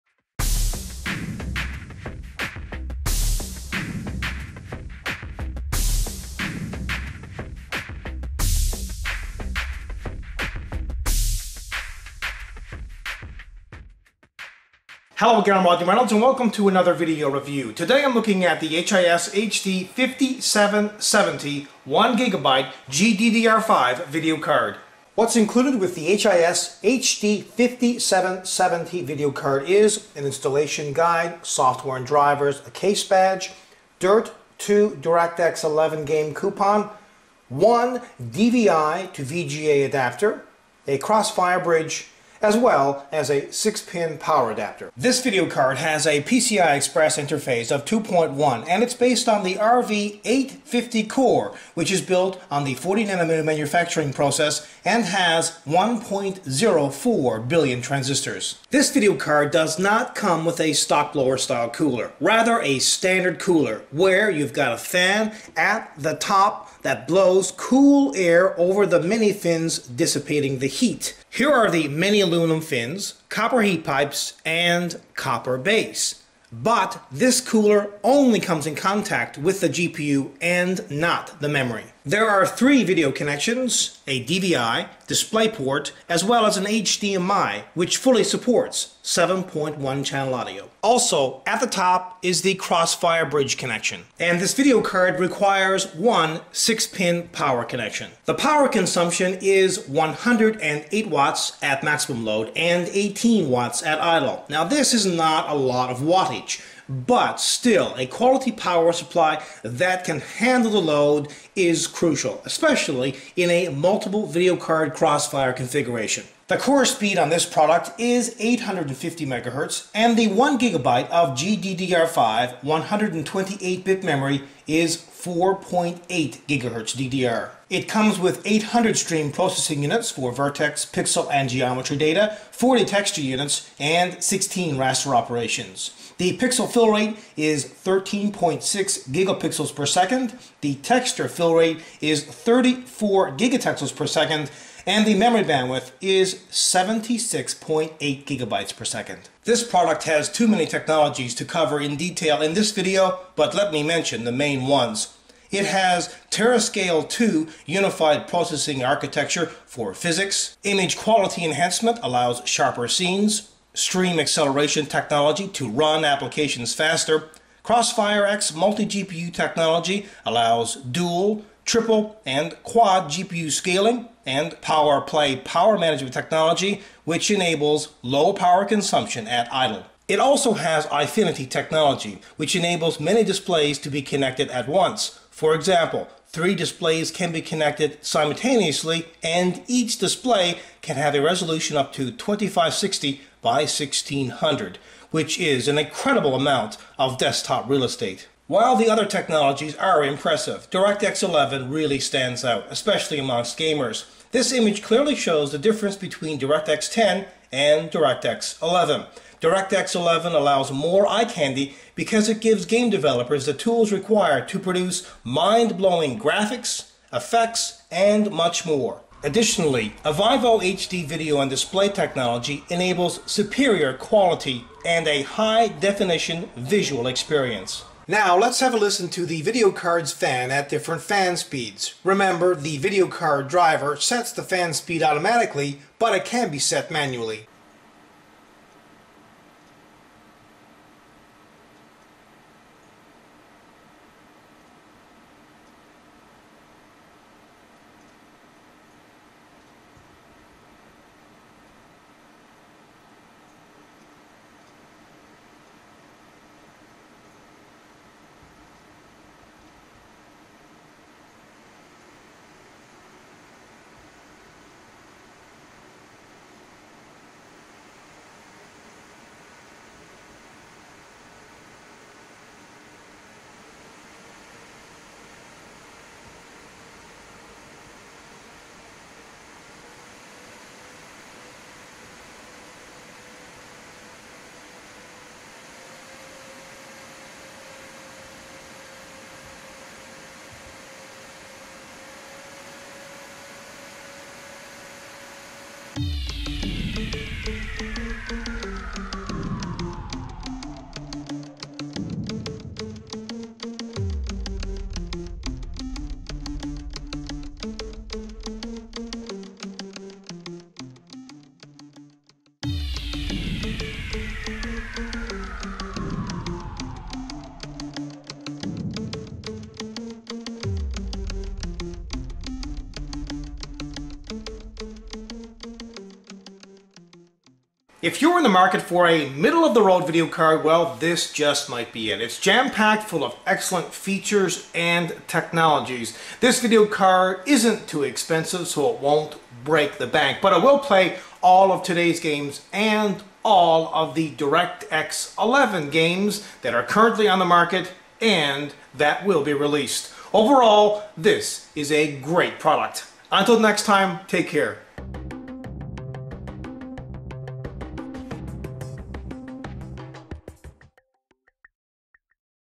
#1139 - HIS HD 5770 1GB GDDR5 Video Card Video Review
The core speed on HIS HD 5770 1GB GDDR5 Video Card is 850MHz & the 1GB of GDDR5 128 bit memory is 4.8GHz DDR. It comes with 800 stream processing units for vertex, pixel & geometry data, 40 texture units & 16 raster operators. The pixel fillrate is 13.6 Gpixels/sec, texture fillrate is 34 GTexels/s & the memory bandwidth is 76.8GB/s. Please note that the cooler on this video card is not the stock blower style cooler & it only comes in contact with the GPU & not the memory.